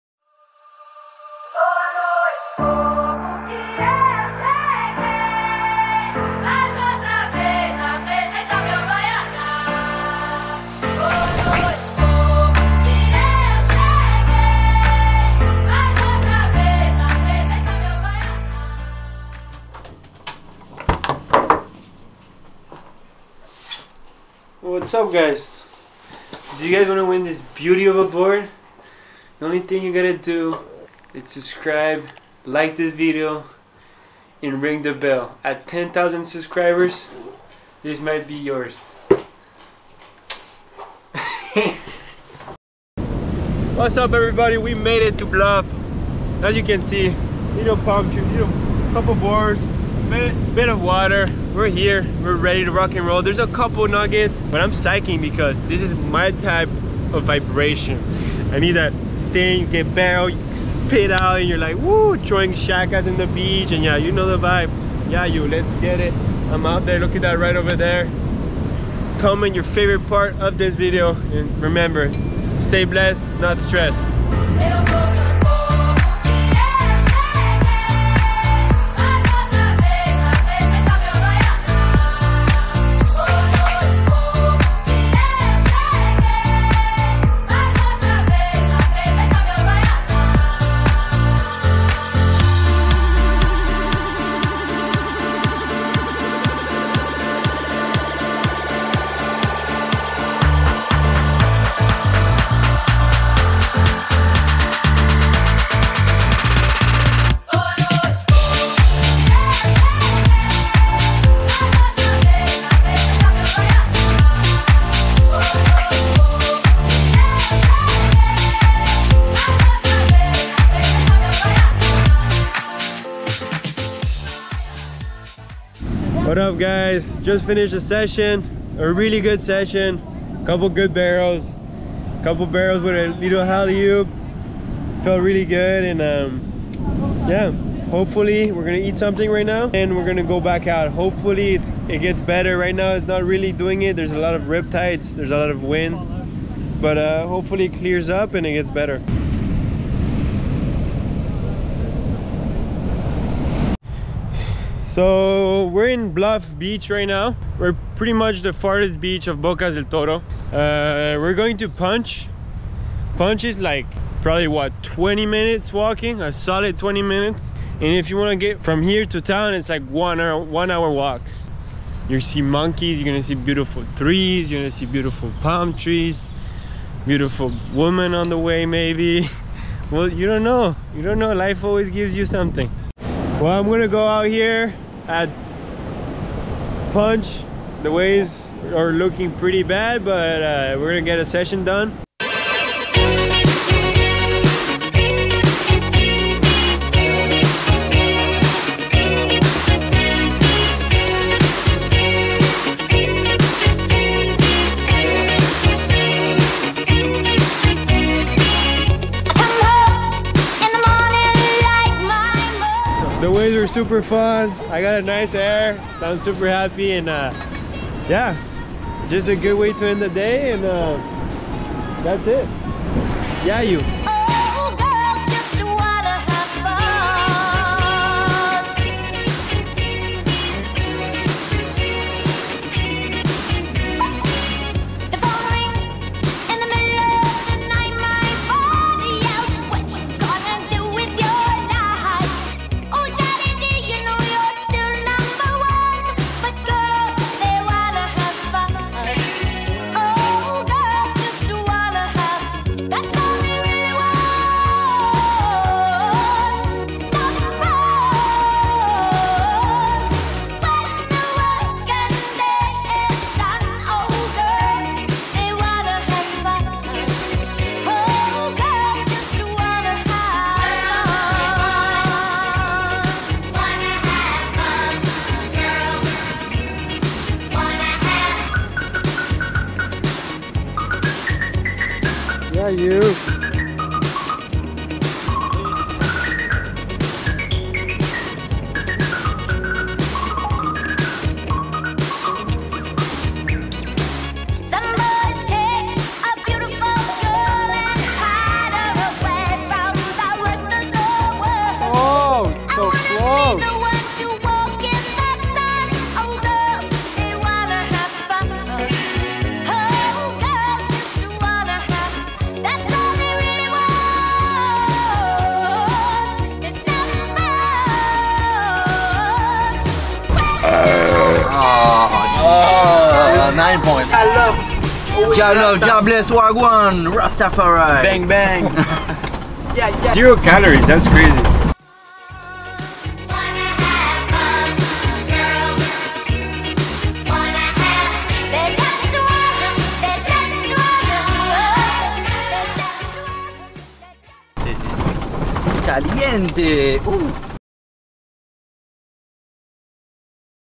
I BROKE MY BOARD SURFING PERFECT BARRELS!! (GIVEAWAY+ LAST PANAMA VLOG)
What was your favorite part? What do you want to see me do? LMK IN THE COMMENTS!

This is the last Panama video! Now it is time to COMPETE!

NEW video next Friday!

This is just the beginning of my new youtube grind (THE GOOD LIFE)! Saludos- Marco Mignot

Shot and edited by Devin Yarde IG- @devin__yarde